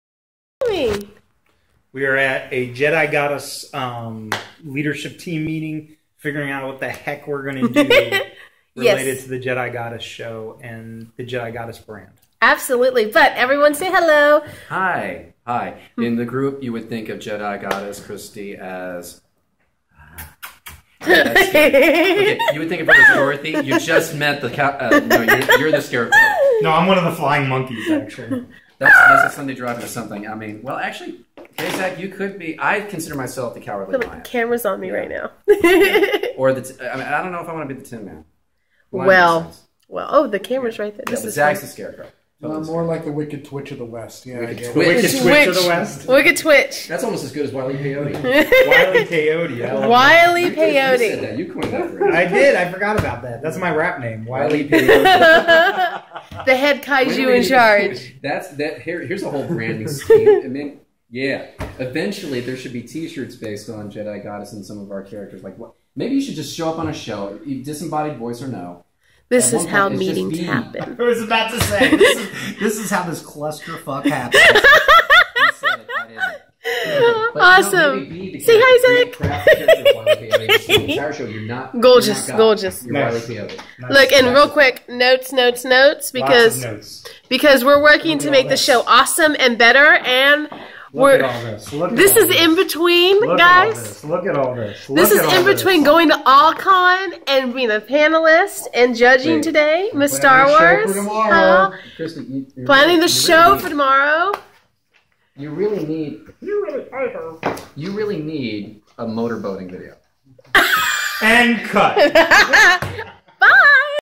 Jedigoddess Presents Show Meeting March 2015
This is what happens when our crazy minds try to plan this show!! Haha Nerdlings unite!! Watch Jedigoddess every Sunday Central time 1245 to 1 45 pm only on deepellumonair.com and jedigoddess.com